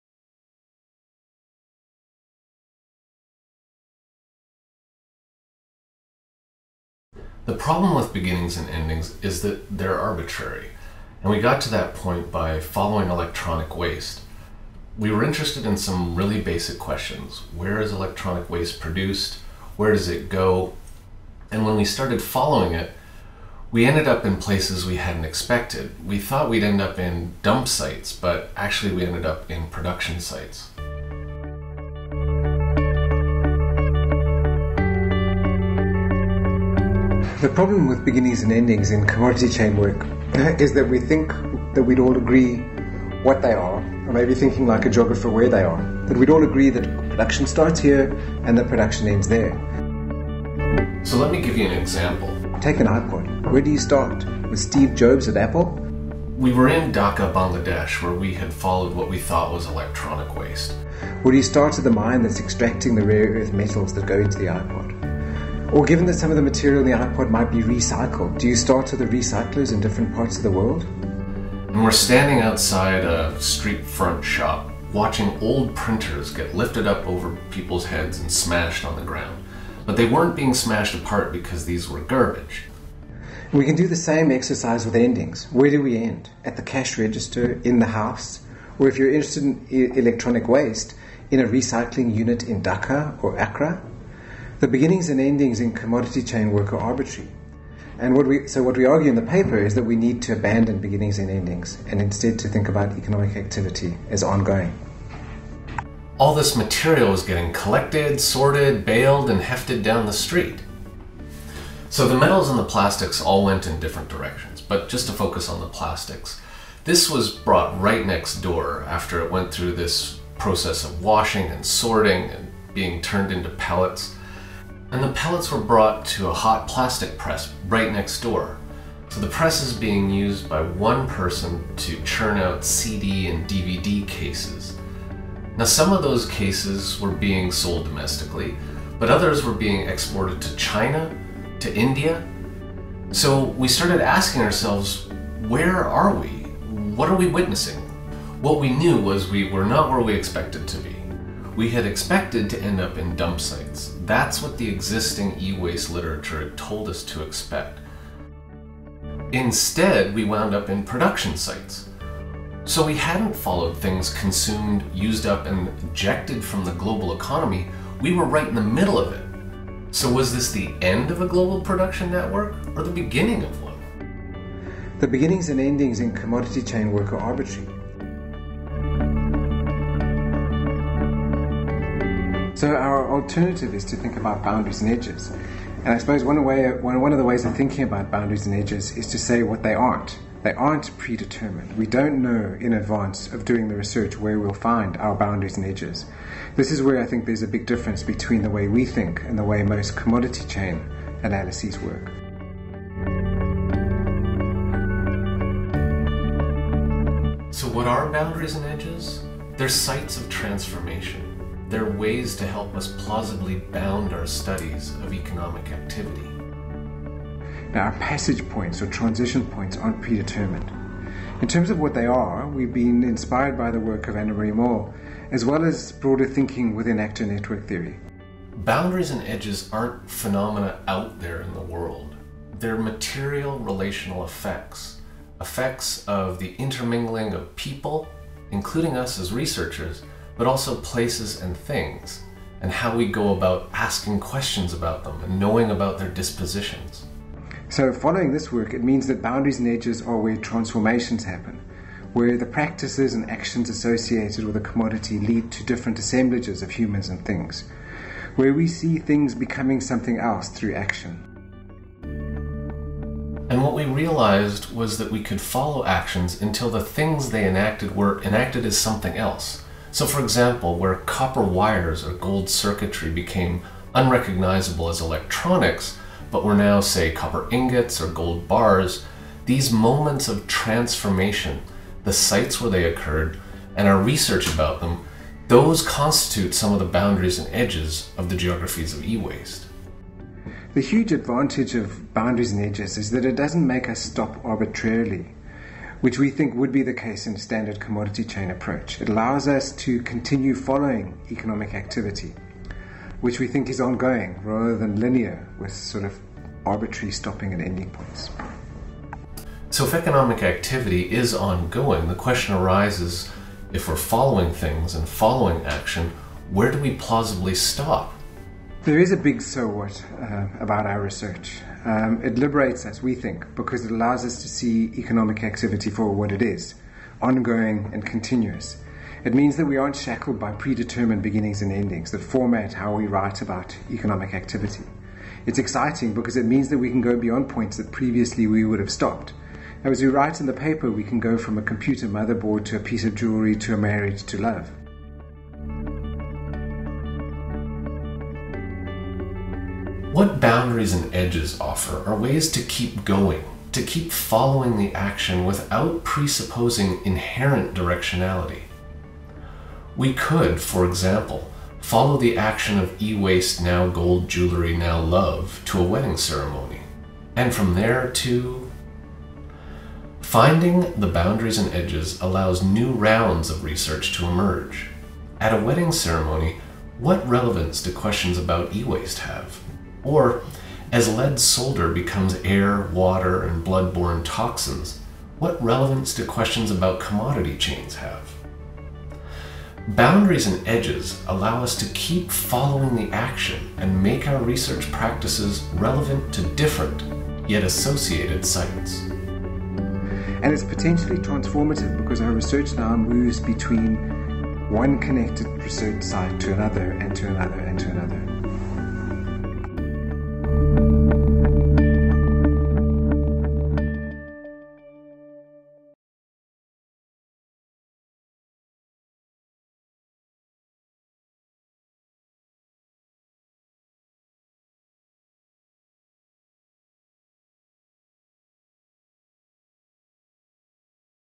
From Beginnings and Endings to Boundaries and Edges
This is a video abstract to accompany the following publication:

Lepawsky, J. and Mather, C. (2011), From beginnings and endings to boundaries and edges: rethinking circulation and exchange through electronic waste. Area, 43: 242--249. doi: 10.1111/j.1475-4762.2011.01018.x